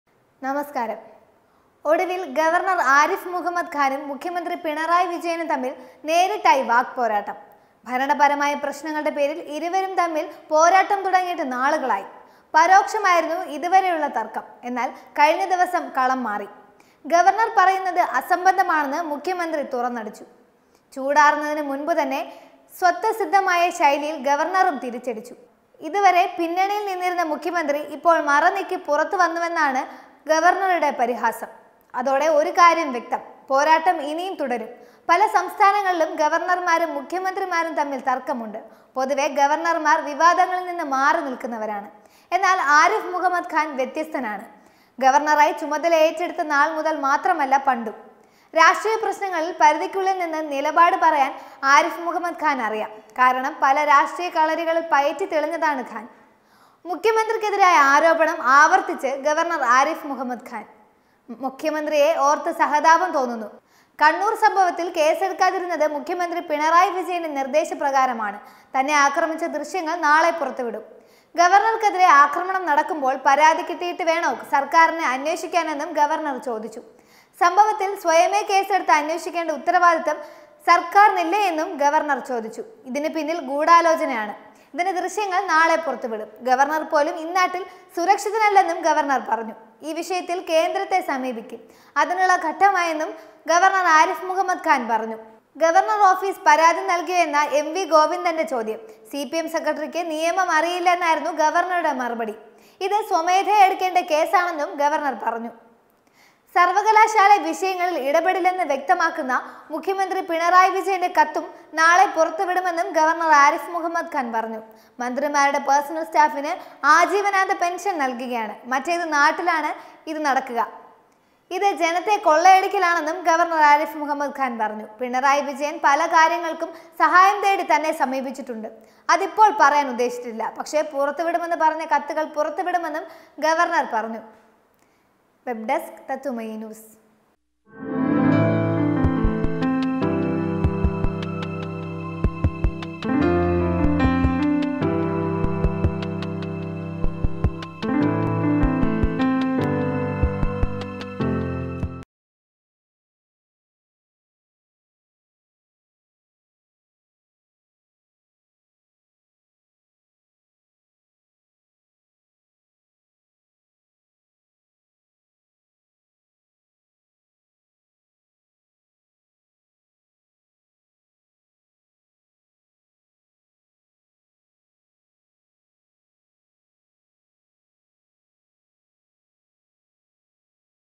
മുഖ്യമന്ത്രിയോട് കൊമ്പുകോർത്ത് ഗവർണർ| ARIFMOHAMMADKHAN
തന്നെ ആക്രമിച്ച ദൃശ്യങ്ങൾ നാളെ പുറത്തുവിടുമെന്ന് ആരിഫിമുഹമ്മദ് ഖാൻ